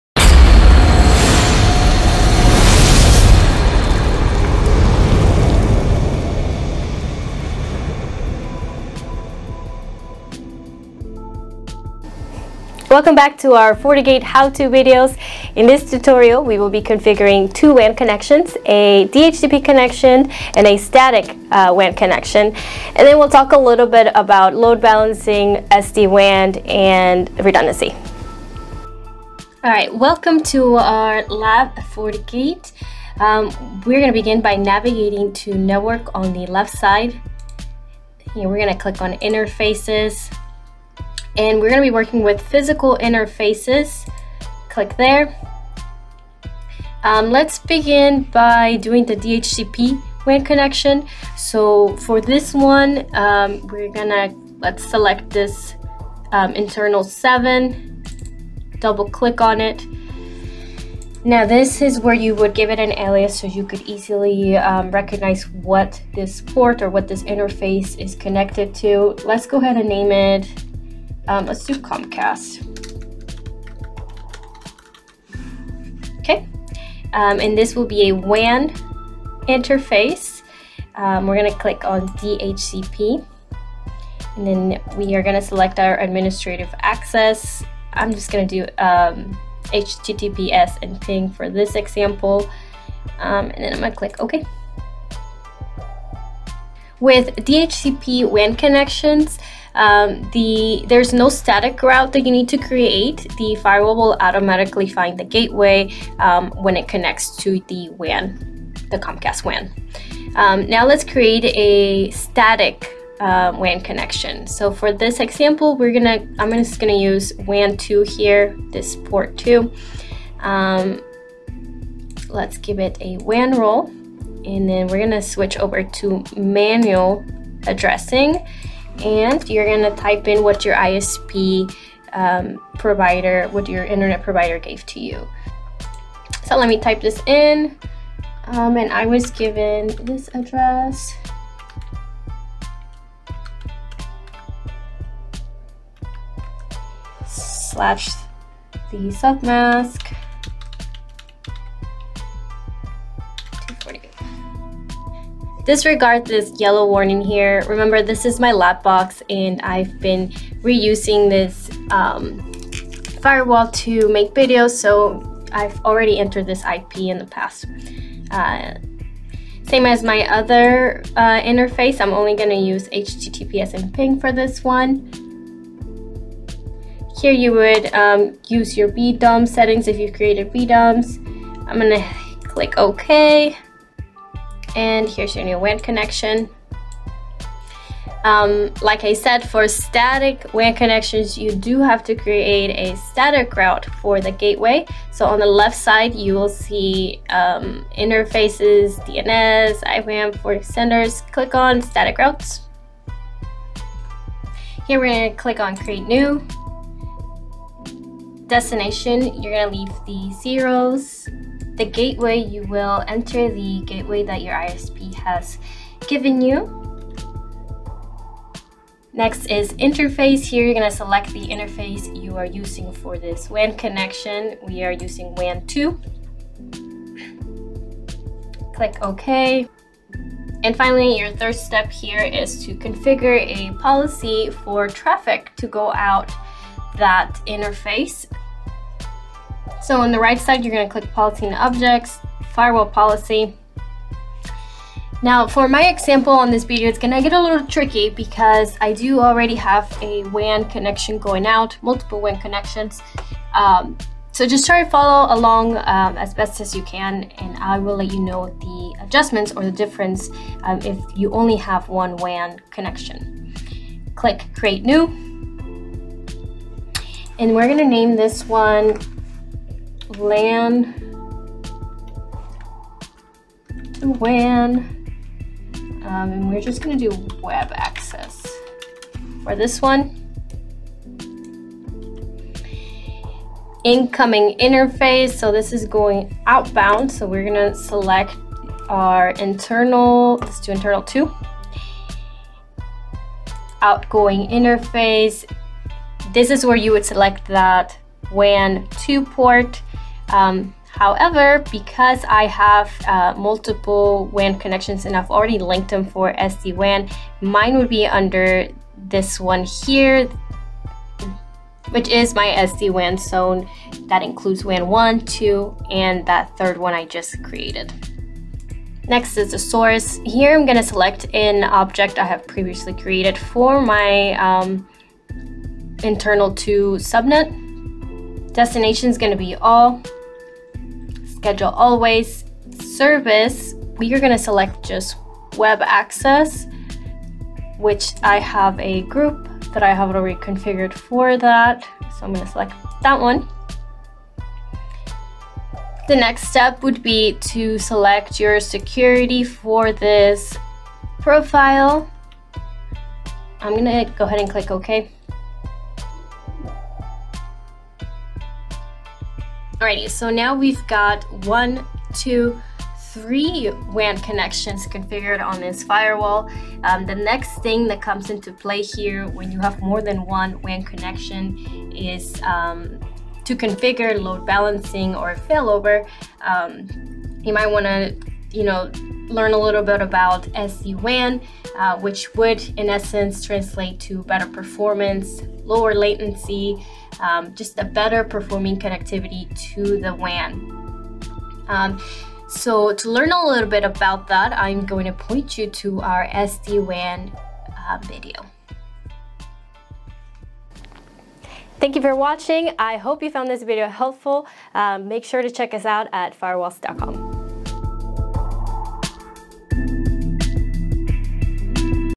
Fortinet: How to Configure a WAN Connection on your FortiGate Firewall
In this Fortinet tutorial, network engineer Jo demonstrates how to configure two different WAN connections on your firewall, as well as a DHCP and Static WAN connection. Jo also discusses load balancing, SD WAN and redundancy.